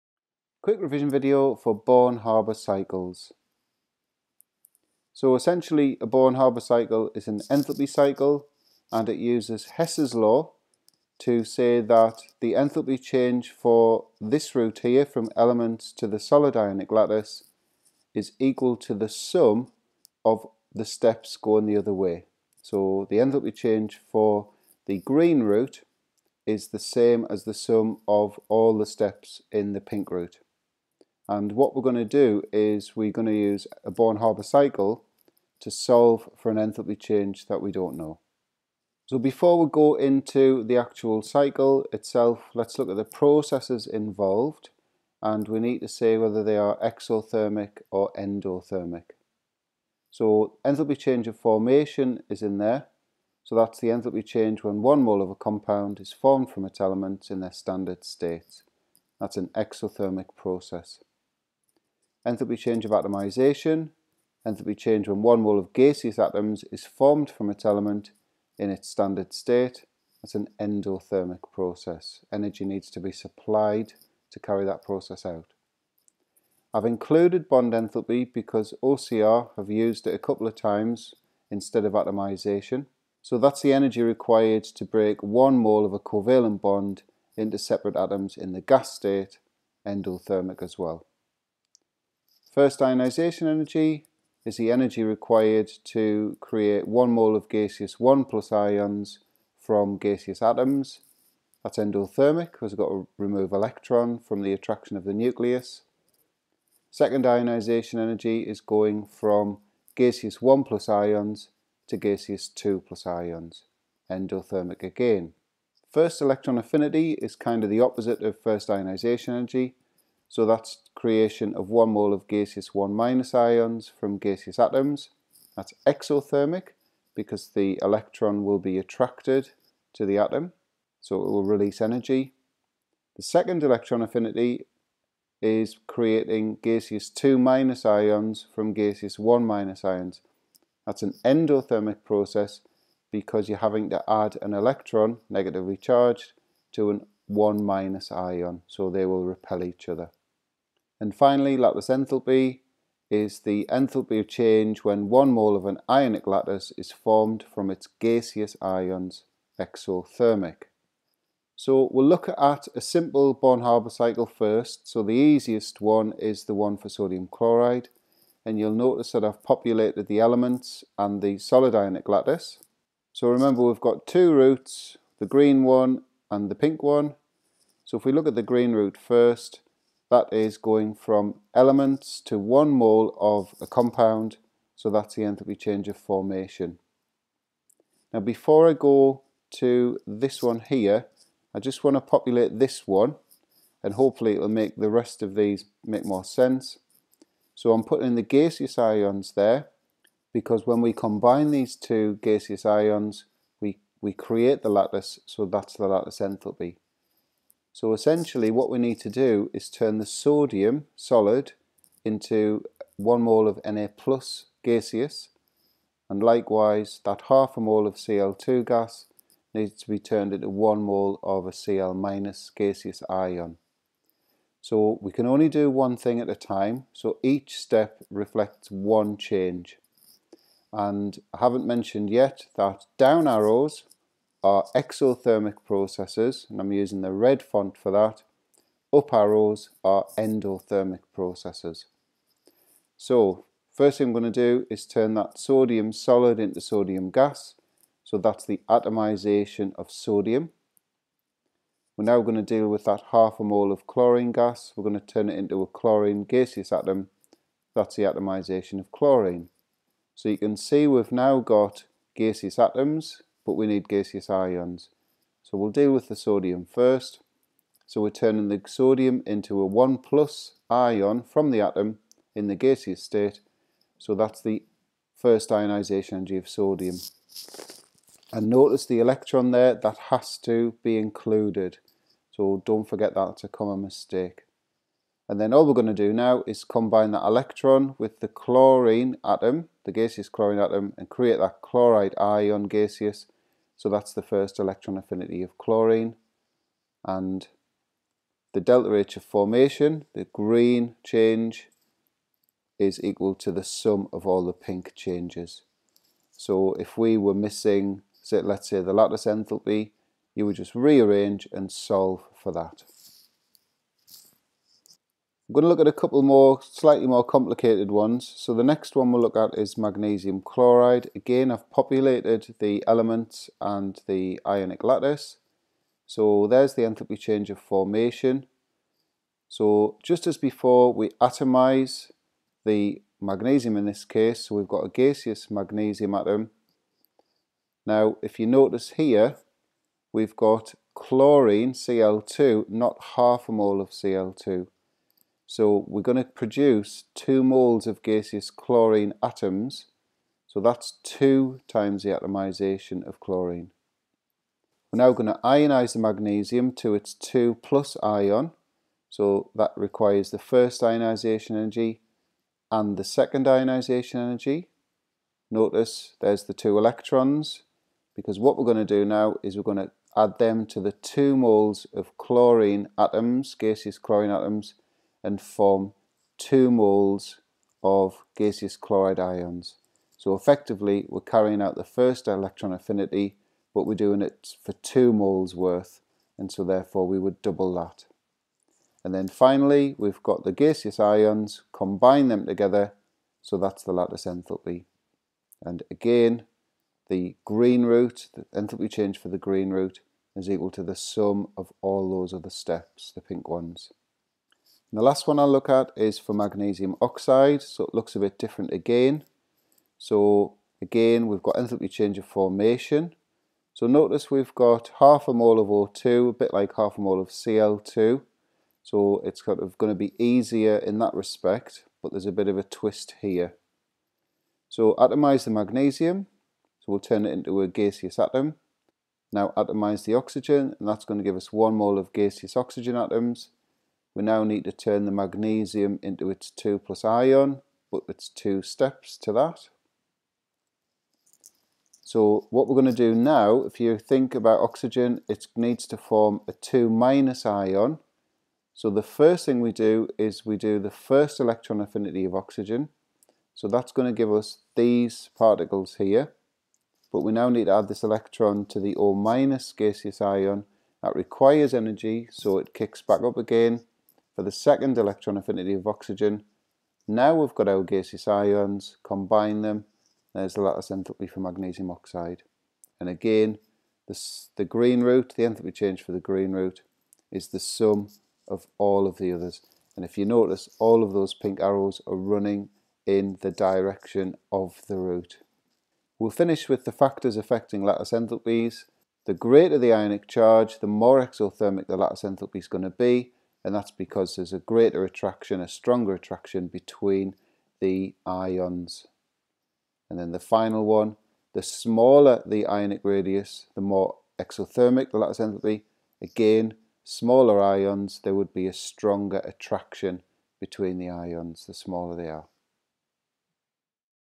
Quick Revision - Born-Haber Cycles
Quick blast through the essentials of Born-Haber cycles followed by a look at those for sodium chloride, magnesium chloride and magnesium oxide.